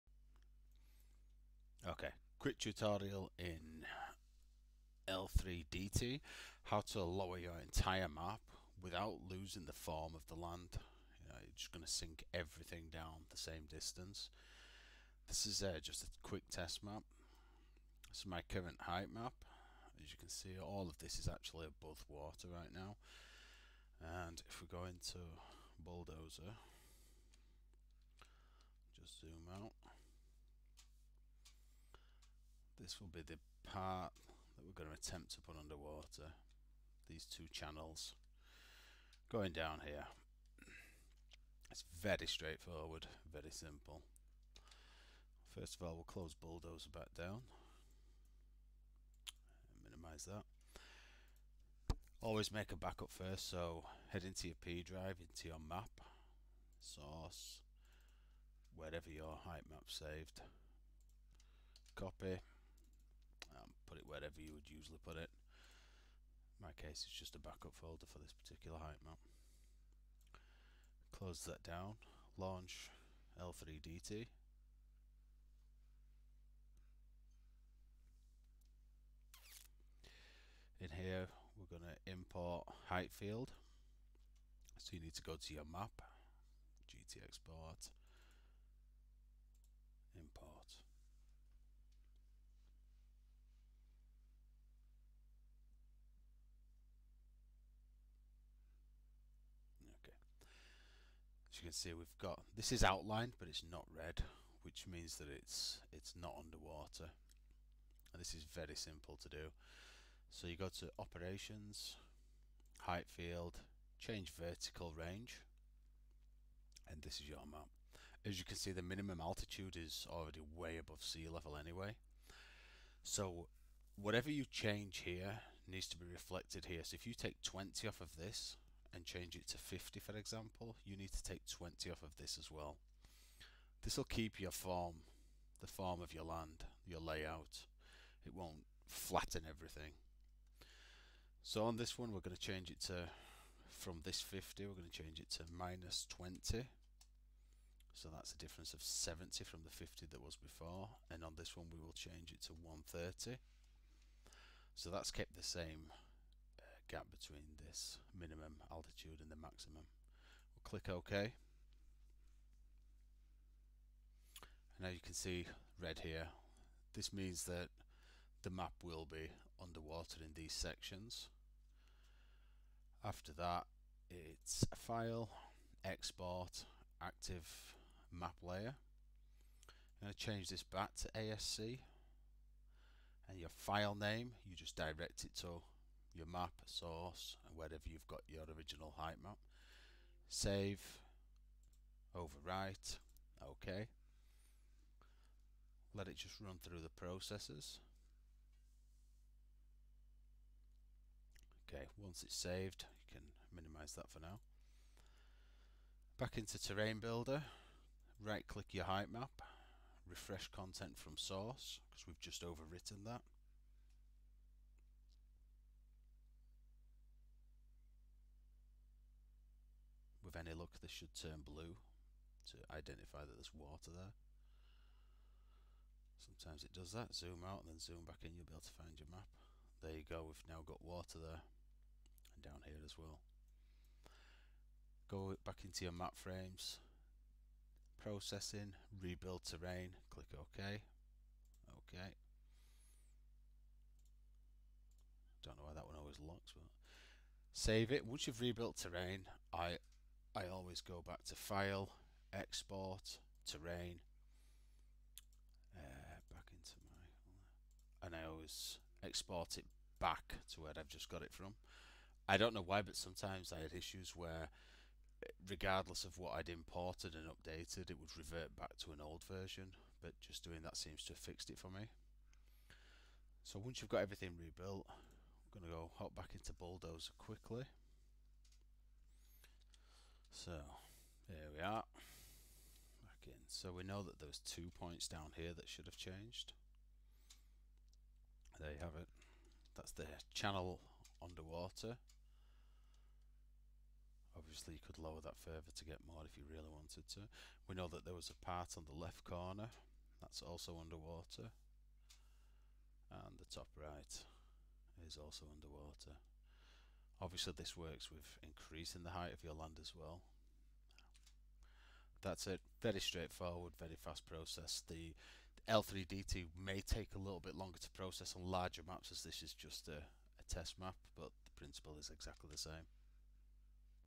Lower Dayz Map in L3DT
Quick tutorial on lowering/raising the whole terrain of Dayz in L3DT.

Thanks to Bullseye who originally talked me through the process!